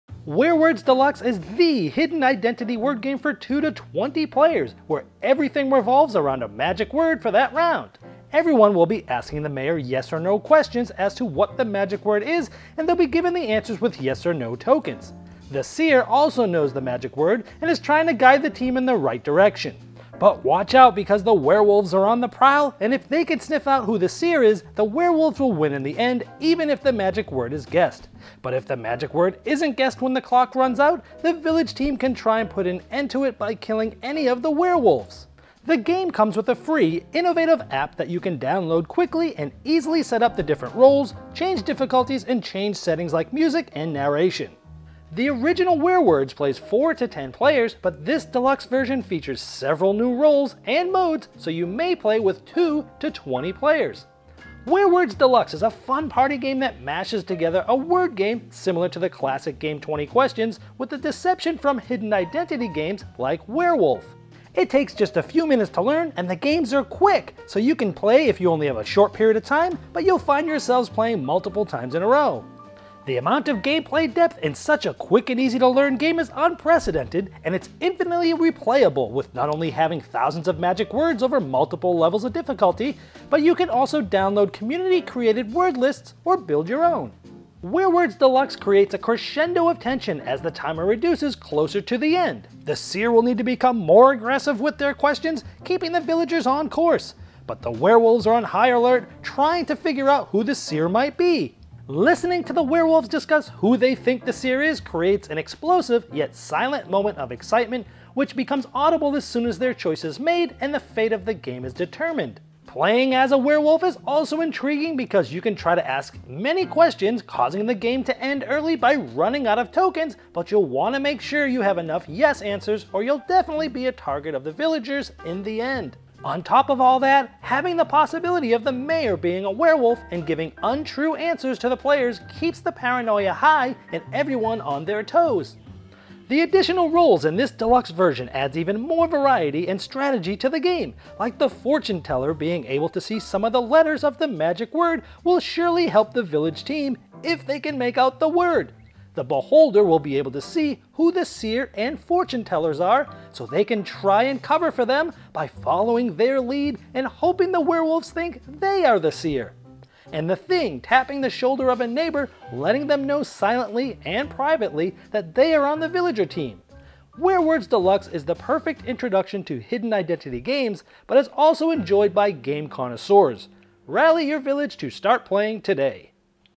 Werewords Deluxe Quick Overview (Bezier Games)
This is a quick 2-minute overview of how Werewords Deluxe works and what makes the game fun!